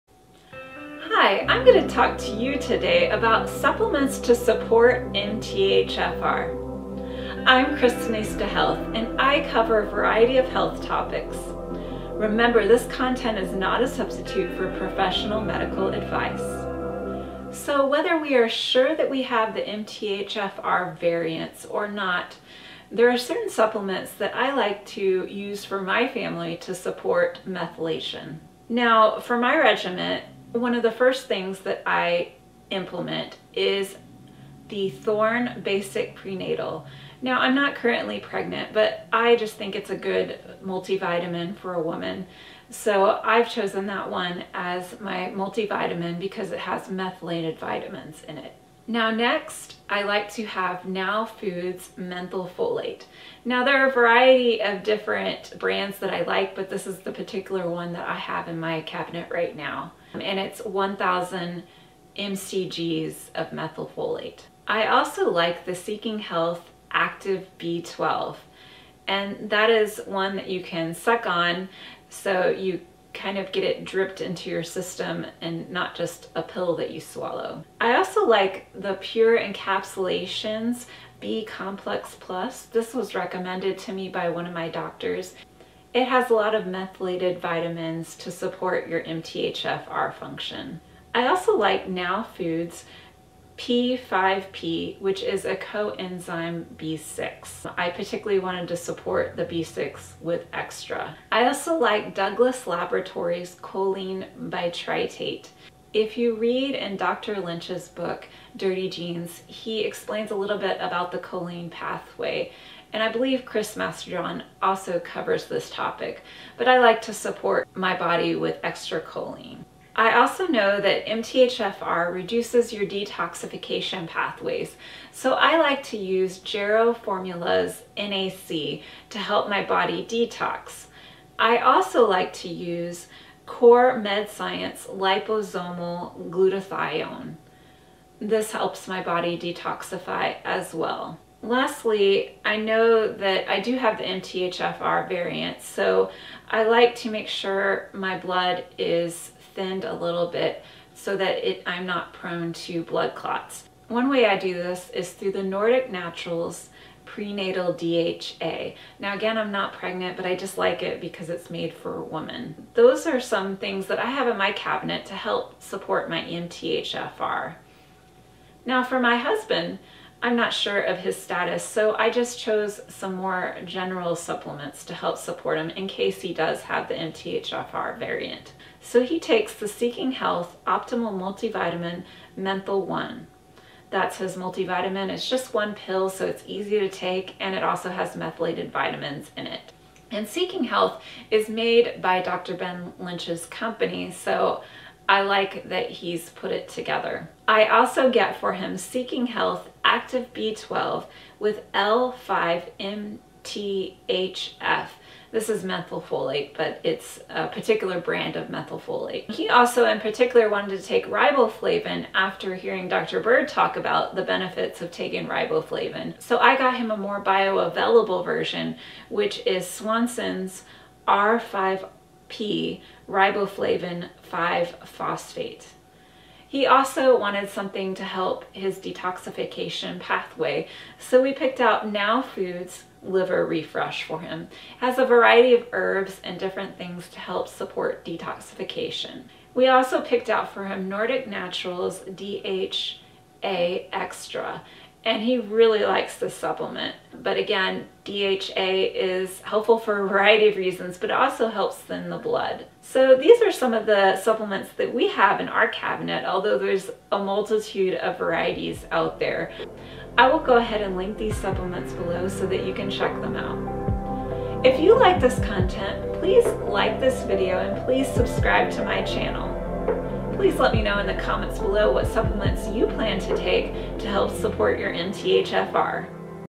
Supplements to Support MTHFR: Methylfolate, NAC, B12, Fish Oil, and more!
Supplements to Support MTHFR: Methylfolate, NAC, B12, Fish Oil, and more!  Have you heard that you have the MTHFR gene mutation, either MTHFR A1298C or C677T, and you aren't sure what to do.  Here I share my MTHFR treatment plan for my family on supporting methylation, helping to put epigenetics at play.

Thorne Basic Prenatal

Now Foods Methyl Folate

Pure Encapsulations B-Complex Plus

Now Foods P-5-P

Douglas Laboratories Choline Bitartrate

Jarrow N-A-C Sustain

Core Med Science Liposomal Glutathione

Nordic Naturals Prenatal DHA

Swanson R-5-P Riboflavin-5-Phosphate

Now Foods Liver Refresh

Nordic Naturals DHA Xtra

0:00 Intro
0:28 Thorne Basic Prenatal
0:48 Now Foods Methyl Folate
1:04 Seeking Health Active B12
1:18 Pure Encapsulations B-Complex Plus
1:31 Now Foods P-5-P
1:41 Douglas Laboratories Choline Bitartrate
1:59 Jarrow N-A-C Sustain
2:11 Core Med Science Liposomal Glutathione
2:21 Nordic Naturals Prenatal DHA
2:58 Seeking Health Optimal Multivitamin Methyl One
3:18 Seeking Health Active B12 with L-5-MTHF
3:29 Swanson R-5-P Riboflavin-5-Phosphate
3:46 Now Foods Liver Refresh
3:59 Nordic Naturals DHA Xtra
4:12 Conclusion

Updated version of camera/lens